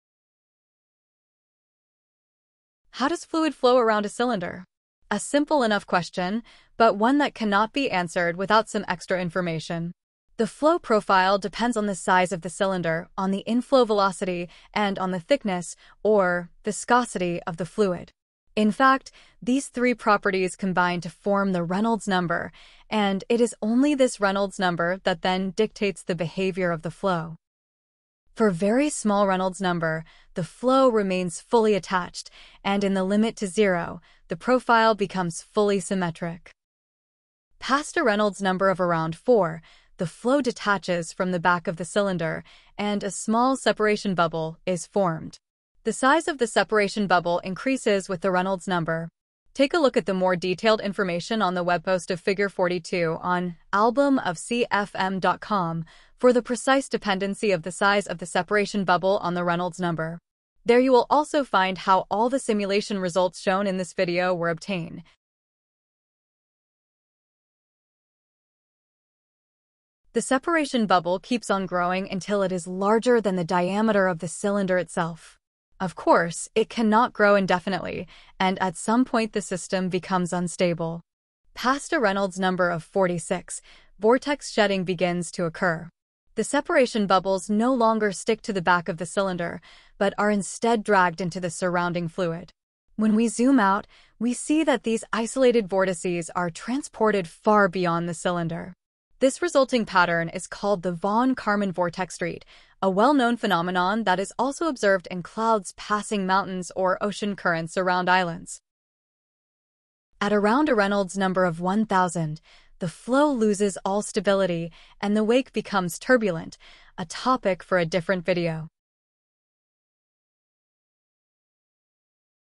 Reynolds number and flow past a cylinder
The seemingly simple system of flow past a cylinder can display a wide variety of flow patterns. The crucial parameter here is the Reynolds number.

A simulation study of a series of figures out of the famous book An Album of Fluid Motion, now redone as An Album of Computational Fluid Motion